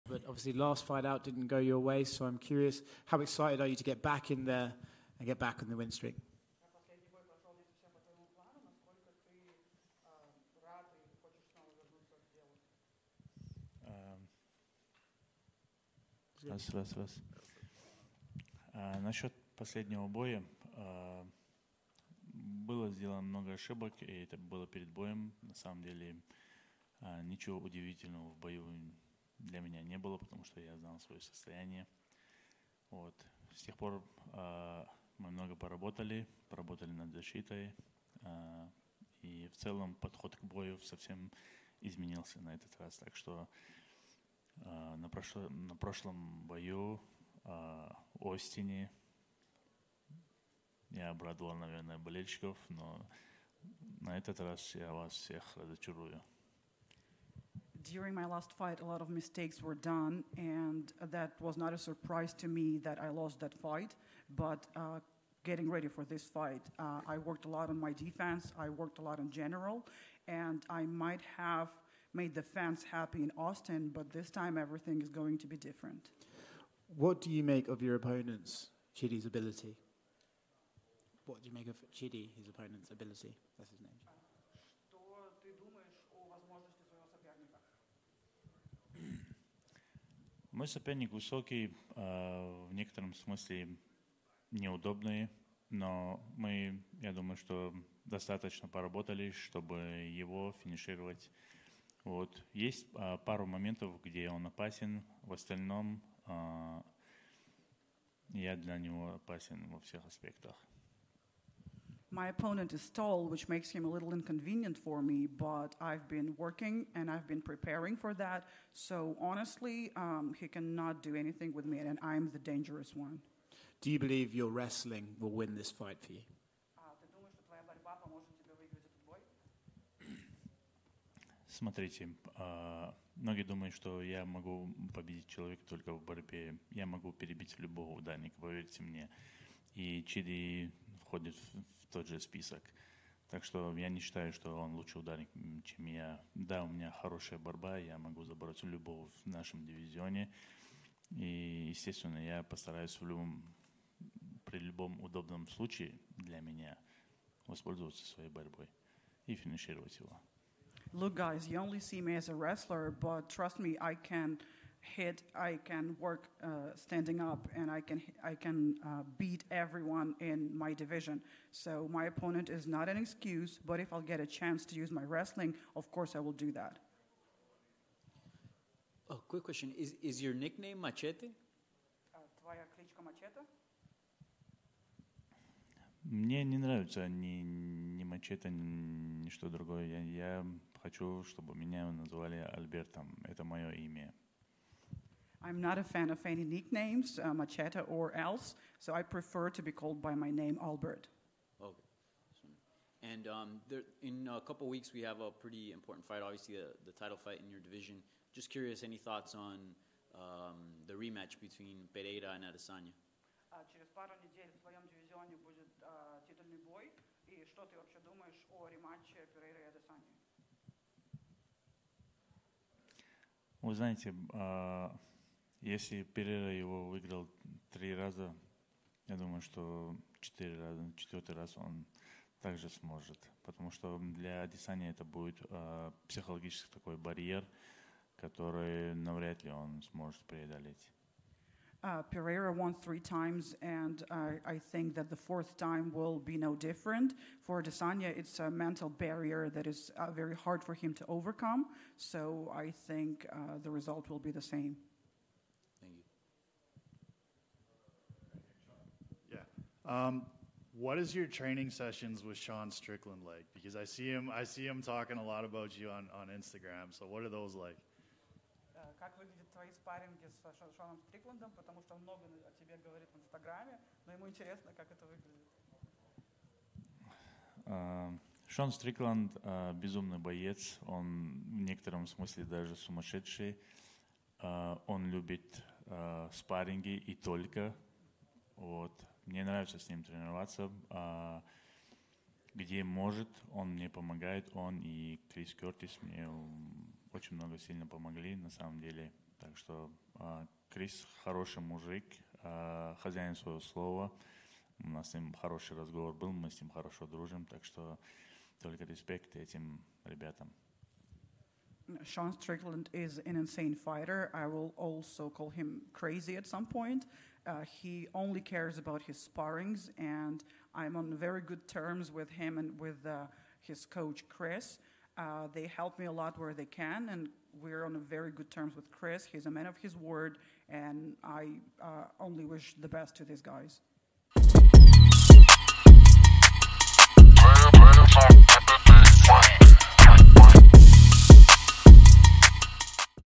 Albert Duraev Media Day | UFC San Antonio
UFC middleweight Albert Duarev discusses his upcoming fight with Chidi Njokuani at UFC San Antonio on March 25.